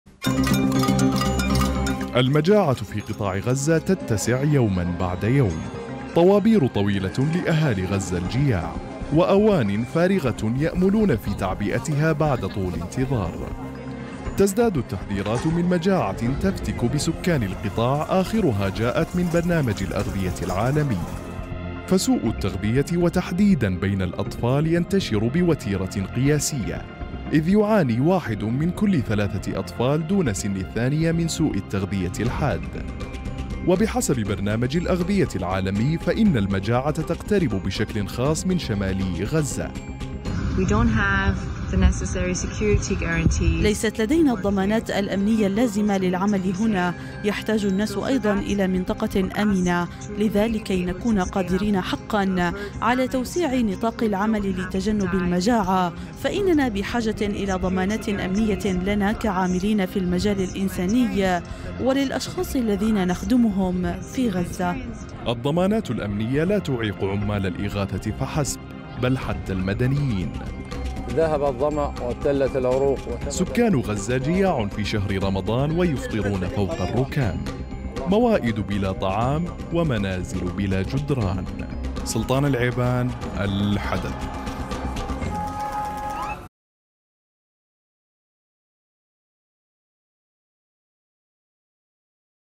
برنامج الأغذية العالمي يحذر من تفاقم المجاعة في قطاع غزة
"1 من كل 3 أطفال يعاني من سوء التغذية".. برنامج الأغذية العالمي يحذر من تفاقم المجاعة في قطاع
لمعرفة أحدث المستجدات.. عبر البث المباشر من هنا ⤵
وبإمكانكم متابعة الحدث على منصات التواصل